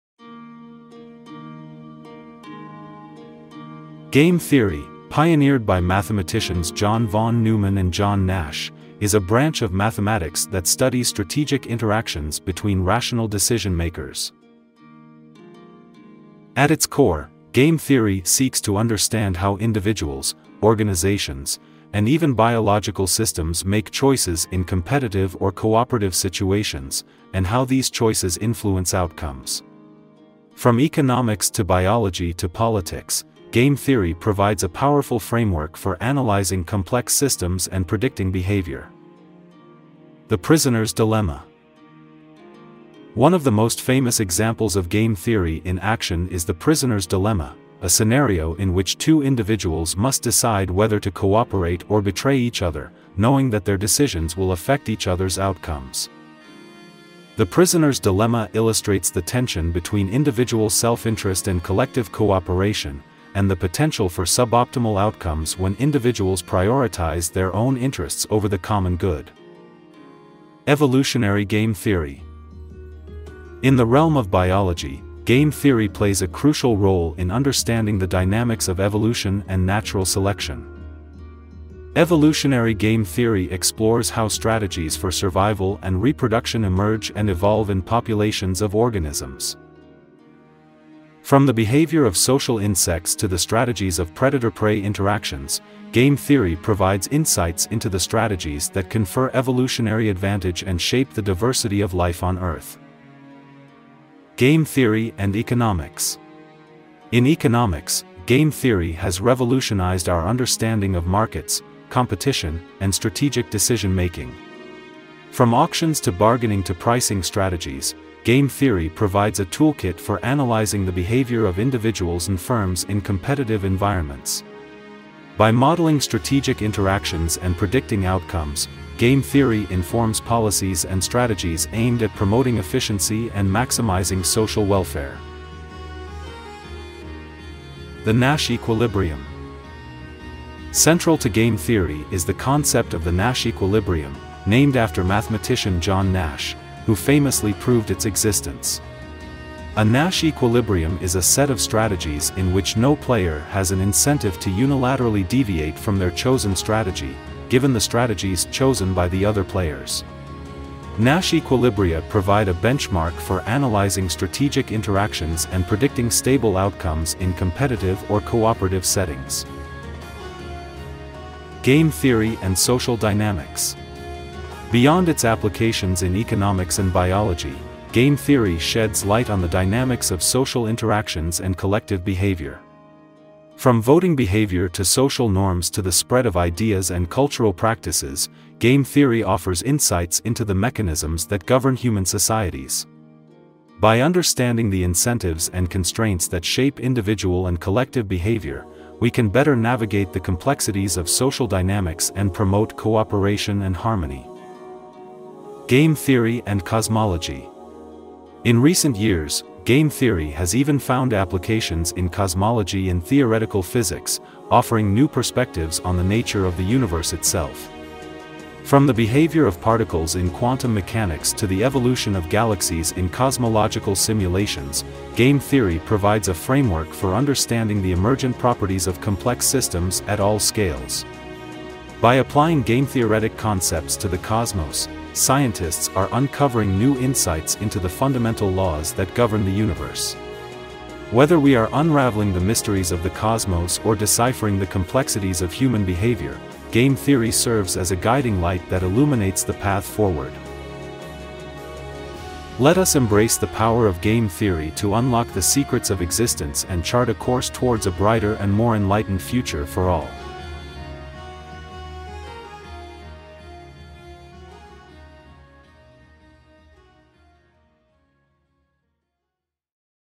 ✨ The Power of Game Theory: Strategies That Shape Our World
🎯 Game theory, pioneered by John von Neumann and John Nash, is the study of strategic interactions among rational decision-makers. From economics to biology and even cosmology, this mathematical framework provides invaluable insights into competitive and cooperative behavior.

🔑 Key Insights:

🤝 The Prisoner's Dilemma: A classic example of cooperation vs. self-interest in decision-making.

🌿 Evolutionary Game Theory: Explains survival and reproduction strategies in nature.

💰 Economic Applications: Helps optimize pricing, auctions, and market strategies.

📊 Nash Equilibrium: The point where no player benefits from changing their strategy alone.

🏛️ Social Behavior: Game theory sheds light on social norms, voting, and cultural practices.

🌌 Game Theory in Cosmology: Even the universe can be analyzed using strategic interactions.

🌍 Whether in business, biology, or the cosmos, game theory offers powerful tools to navigate complexity and drive better decision-making.

🎥 This video visually includes: Engaging visuals illustrating strategic decision-making in economics, biology, and everyday life through real-world examples and dynamic animations.

✨ نظرية الألعاب: استراتيجيات تشكل عالمنا

🎯 نظرية الألعاب، التي طورها جون فون نيومان وجون ناش، تدرس التفاعلات الاستراتيجية بين صناع القرار العقلانيين. من الاقتصاد إلى علم الأحياء وحتى علم الكونيات، توفر هذه الأداة الرياضية رؤى قيمة حول السلوك التنافسي والتعاوني.

🔑 النقاط الرئيسية:

🤝 معضلة السجين: مثال كلاسيكي للصراع بين التعاون والمصلحة الذاتية.

🌿 نظرية الألعاب التطورية: تشرح استراتيجيات البقاء والتكاثر في الطبيعة.

💰 التطبيقات الاقتصادية: تساعد في تحسين الأسعار، المزادات، واستراتيجيات السوق.

📊 توازن ناش: النقطة التي لا يستفيد فيها أي لاعب من تغيير استراتيجيته بمفرده.

🏛️ السلوك الاجتماعي: تسلط الضوء على العادات الاجتماعية، التصويت، والممارسات الثقافية.

🌌 نظرية الألعاب وعلم الكون: حتى الكون يمكن تحليله من خلال التفاعلات الاستراتيجية.

🌍 سواء في الأعمال التجارية أو الطبيعة أو الكون، توفر نظرية الألعاب أدوات قوية لفهم التعقيدات وتحسين اتخاذ القرار.

📌 تواصل معنا:
🌐 الموقع الإلكتروني: aland

🎥 يتضمن هذا الفيديو من الناحية البصرية: رسوم متحركة توضيحية تعرض سيناريوهات اتخاذ القرارات الاستراتيجية في الاقتصاد وعلم الأحياء والحياة اليومية.

✨ Spieltheorie: Strategien, die unsere Welt prägen

🎯 Die Spieltheorie, entwickelt von John von Neumann und John Nash, untersucht strategische Interaktionen zwischen rationalen Entscheidungsträgern. Von der Wirtschaft über die Biologie bis hin zur Kosmologie bietet sie wertvolle Einblicke in Wettbewerb und Kooperation.

🔑 Wichtige Erkenntnisse:

🤝 Das Gefangenendilemma: Ein klassisches Beispiel für die Wahl zwischen Kooperation und Eigeninteresse.

🌿 Evolutionäre Spieltheorie: Erklärt Überlebens- und Fortpflanzungsstrategien in der Natur.

💰 Wirtschaftliche Anwendungen: Optimiert Preise, Auktionen und Marktstrategien.

📊 Nash-Gleichgewicht: Der Punkt, an dem kein Spieler von einer Strategieänderung profitiert.

🏛️ Soziales Verhalten: Erhellt soziale Normen, Abstimmungen und kulturelle Praktiken.

🌌 Spieltheorie in der Kosmologie: Selbst das Universum kann durch strategische Interaktionen analysiert werden.

🌍 Ob in Wirtschaft, Natur oder Universum – die Spieltheorie bietet leistungsstarke Werkzeuge zur Bewältigung komplexer Entscheidungen.

🎥 Dieses Video beinhaltet visuell: Dynamische Animationen, die strategische Entscheidungen in Wirtschaft, Biologie und dem Alltag darstellen.